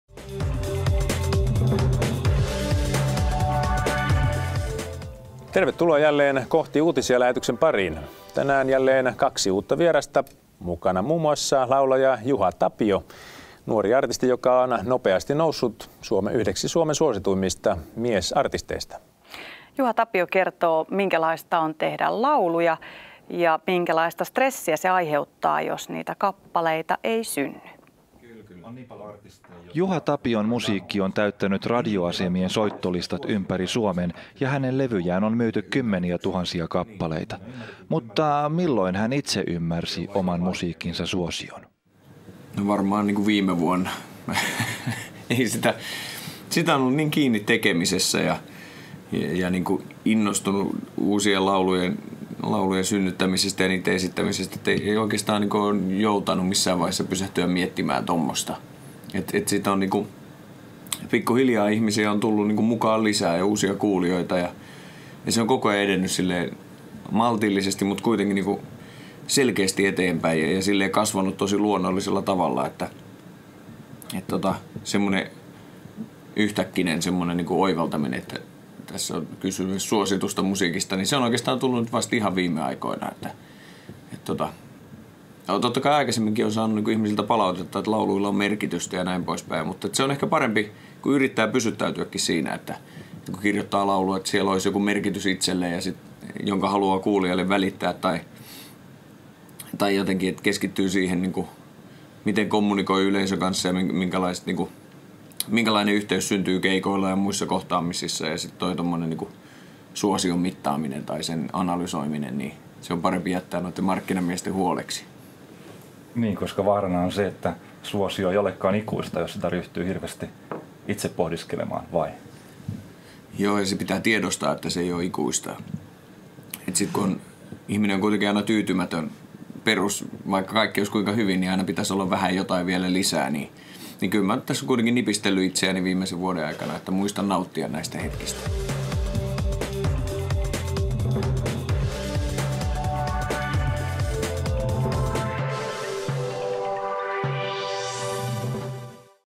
Kohti uutisia: Juha Tapio
Muusikko Juha Tapio pohtii haastattelussamme muun muassa biisien tekemisen vaikeutta.